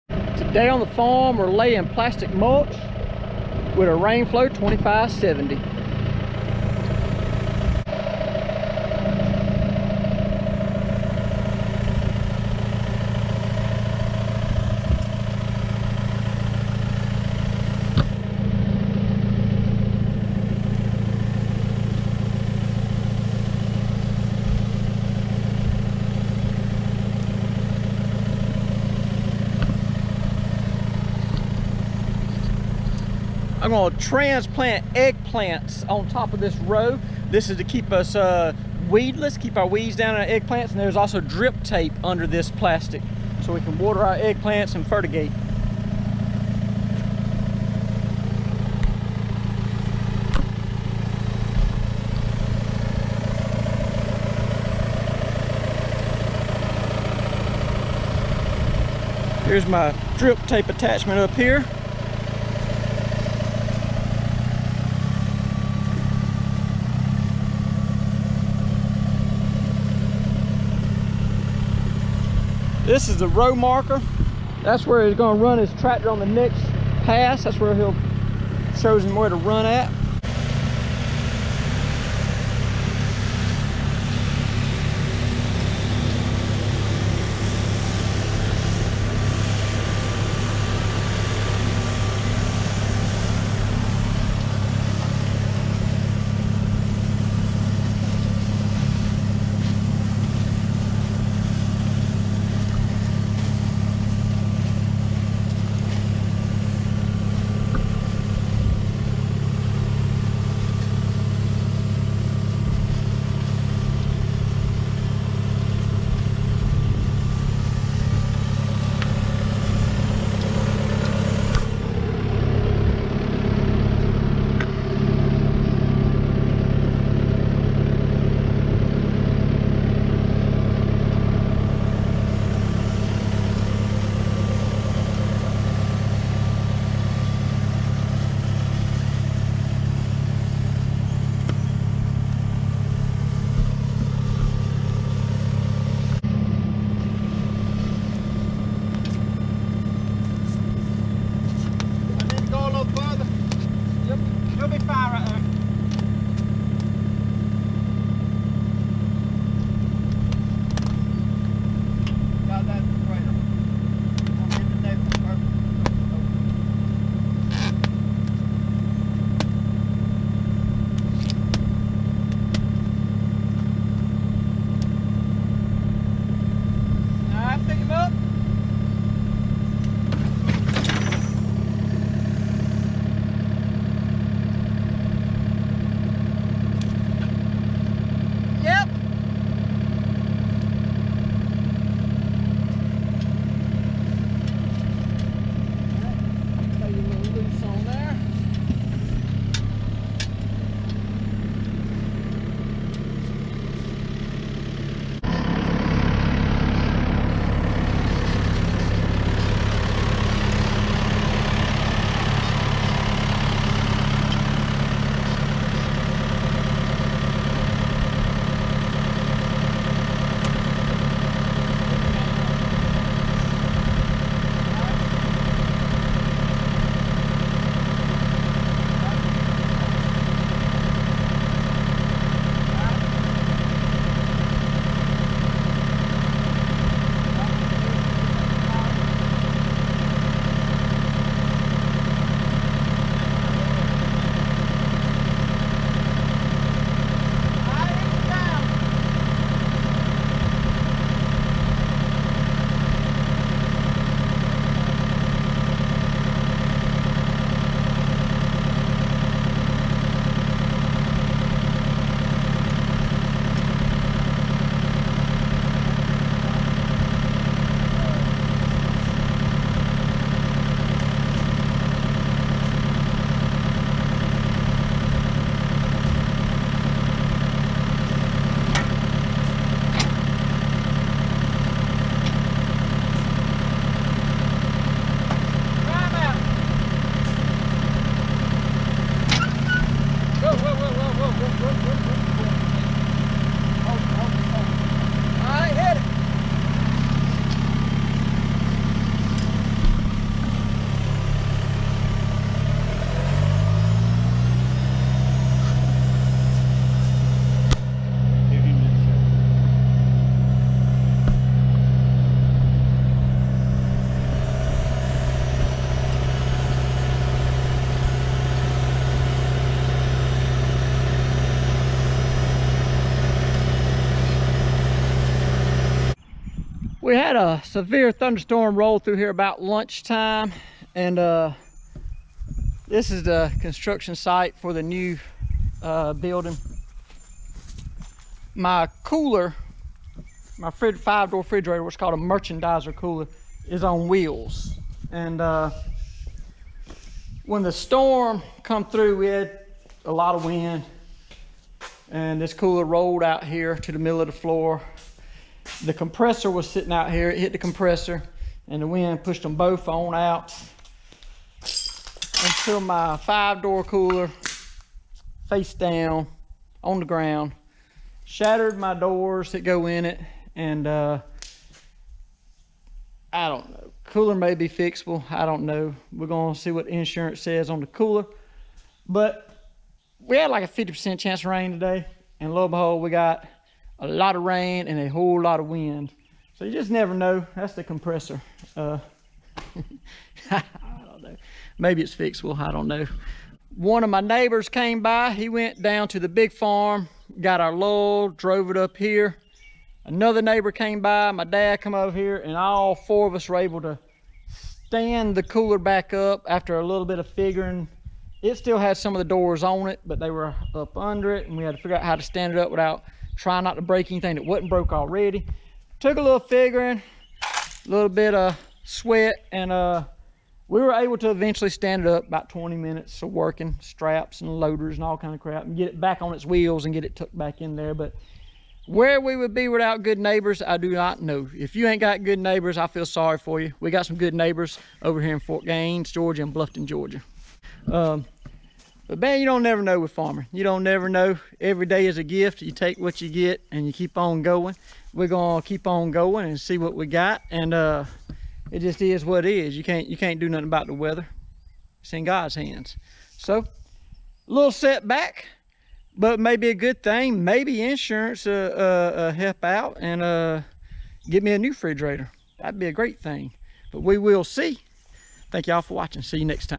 Laying plastic mulch and thunderstorm results
We are laying plastic mulch in the garden and standing my cooler back up after the storm.